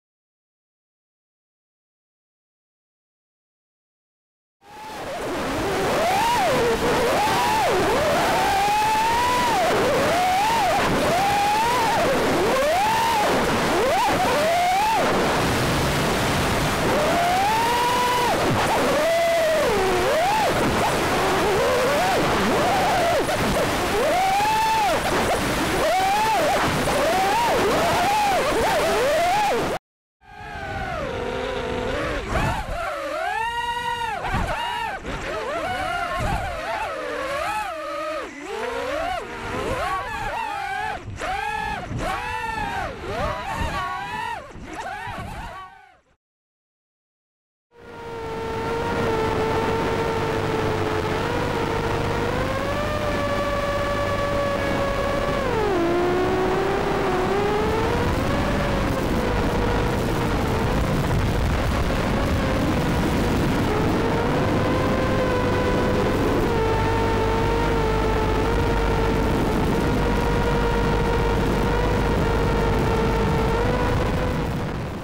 FPV Onboard MIC - Audio Quality Comparison
Comparing the audio quality between
+ Built-in Microphone on VTX - FX795-2
+ External MIC ($2.2 from BG)
+ Gopro recorded audio

The External Mic from banggood actually performed really well!